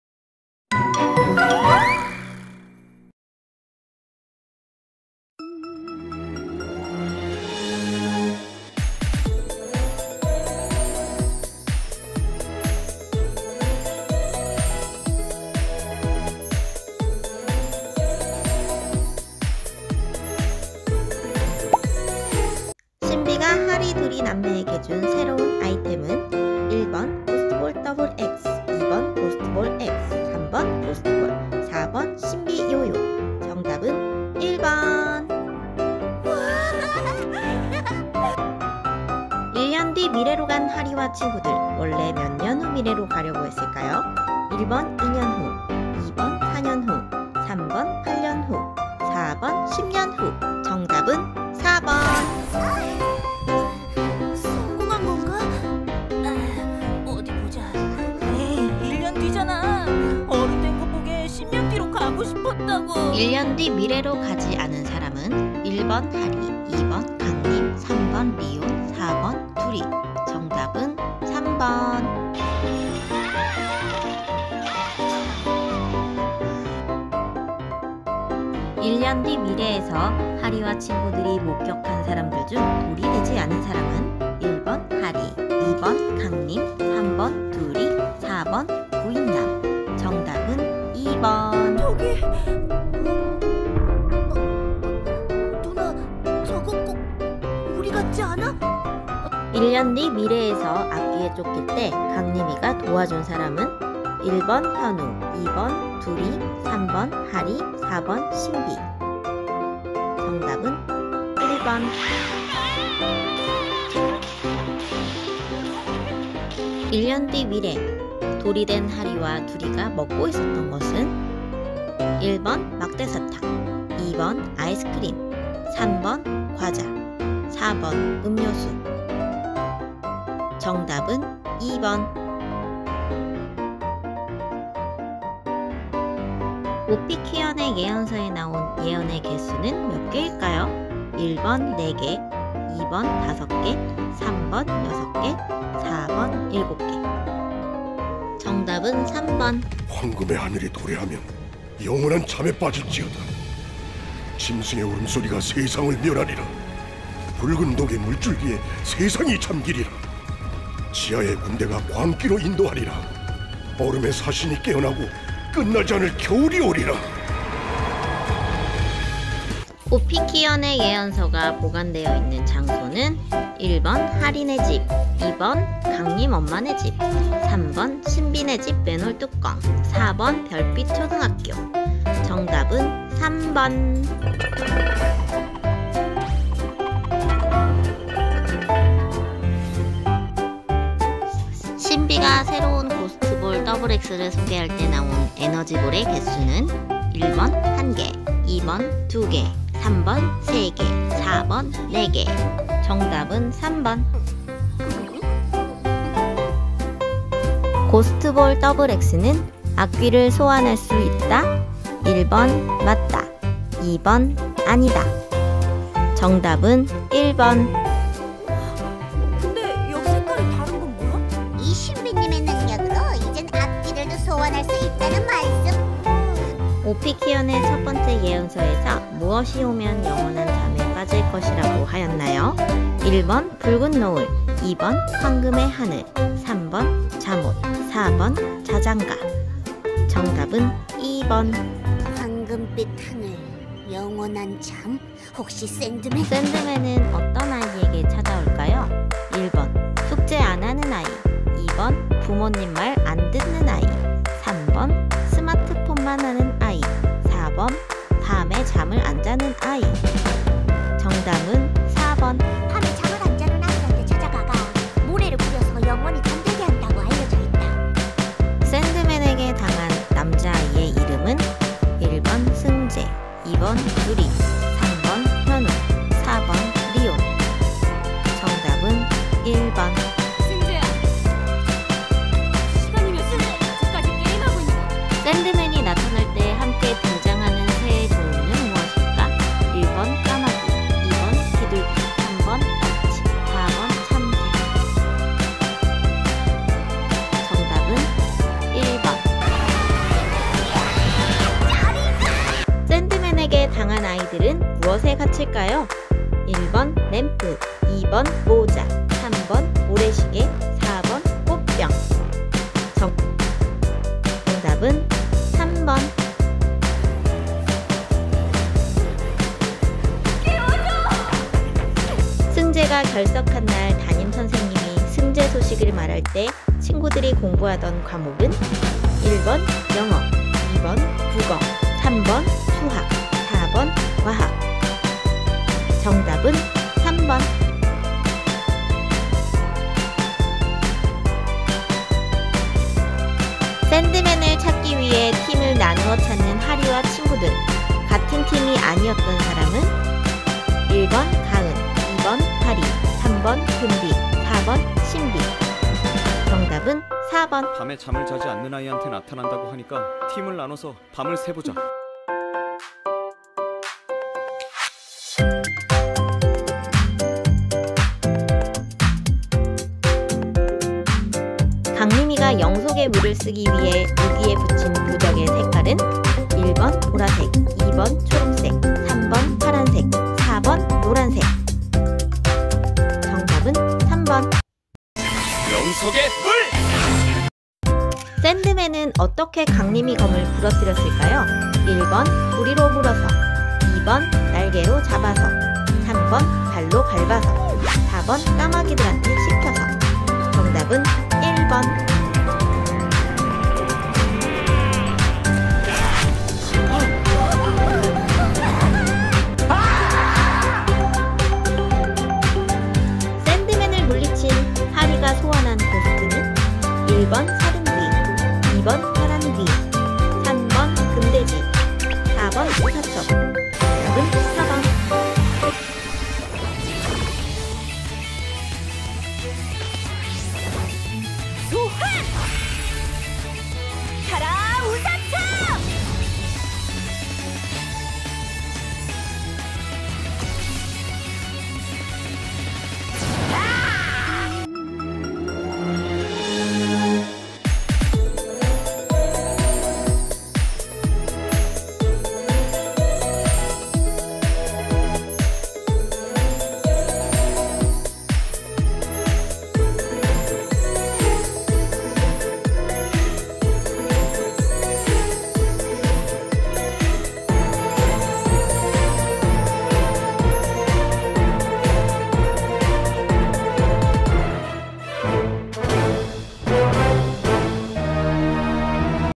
[신비아파트 퀴즈] 신비아파트 퀴즈쇼 1탄 샌드맨
신비아파트 공식 앱에 있는 퀴즈를 풀어보았어요~^^